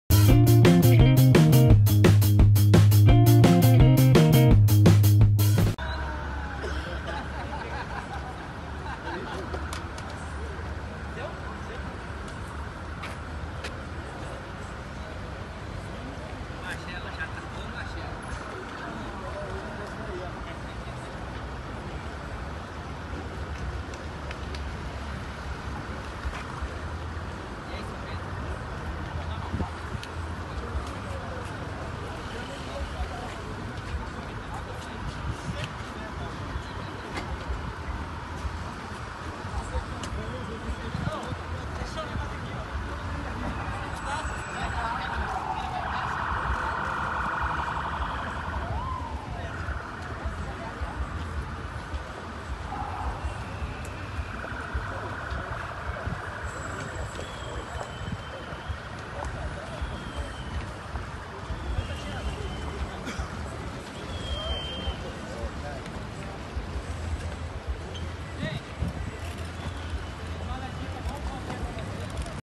Já pensou você chegando na Feira ForMobile de limousine?
Já pensou você chegando na @feiraformobile de limousine? A @metalnox.ferragens em parceria com @wcenter_sp vai levar hoje 60 marceneiros para este grande evento. Confere aí como foi a chegada do primeiro grupo.